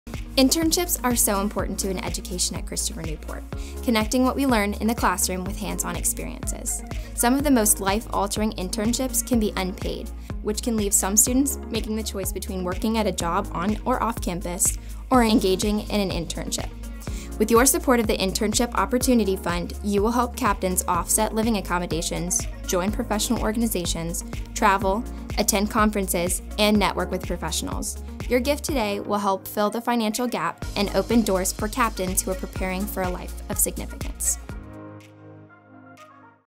The Internship Opportunity Fund
Make it happen for a Captain! Jenna Lowrance ‘19 is employed by the Boys & Girls Clubs of the Virginia Peninsula as a result of an internship she did while at CNU. Your gift to the Internship Opportunity Fund will help more Captains prepare for great careers Thank you!